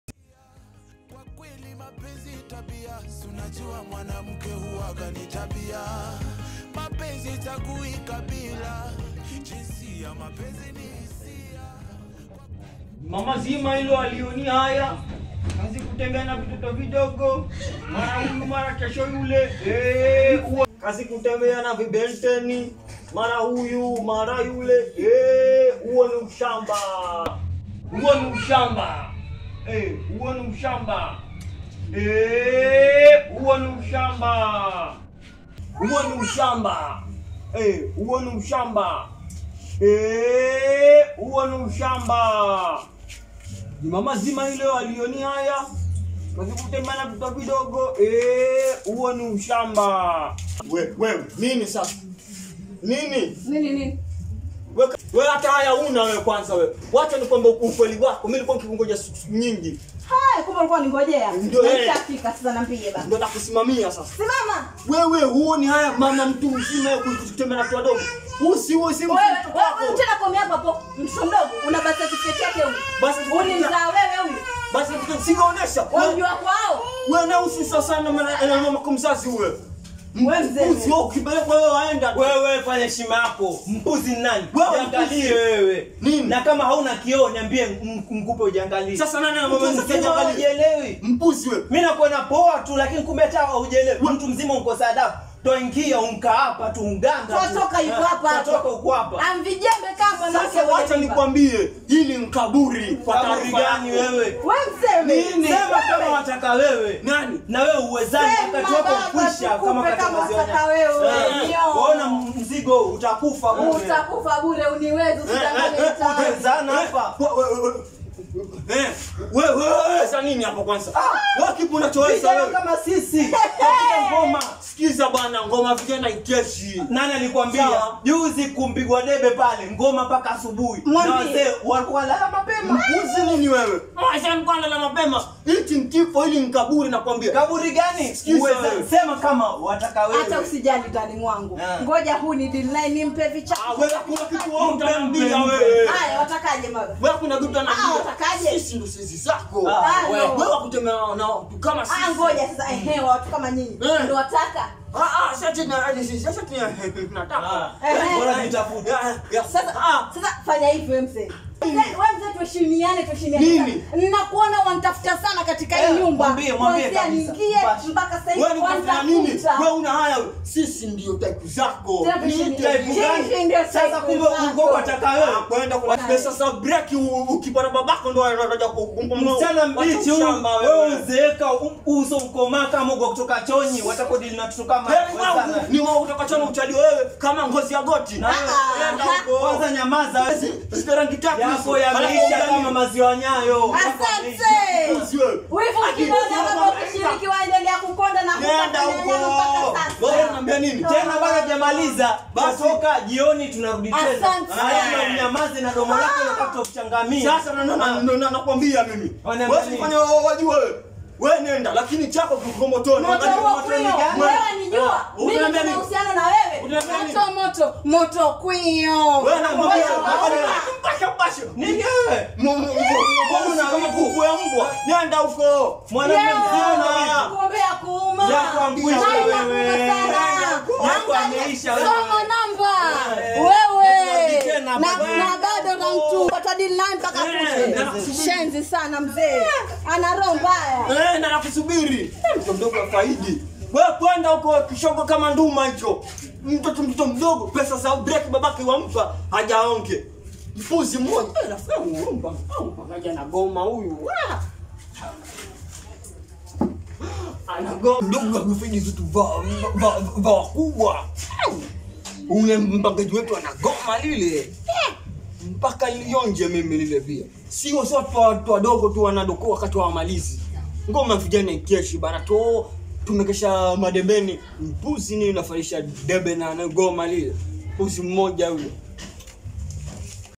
TABIA ZA WAKENYA - WAZEE WA MOMBASA
Kuna wazee wengine bana, wanaudhi hasa akiwa ni Jirani yako alafu hana mke hapo na hataki shikamoo.
Hizi tabia utazipata Sana uswahilini kwetu huku.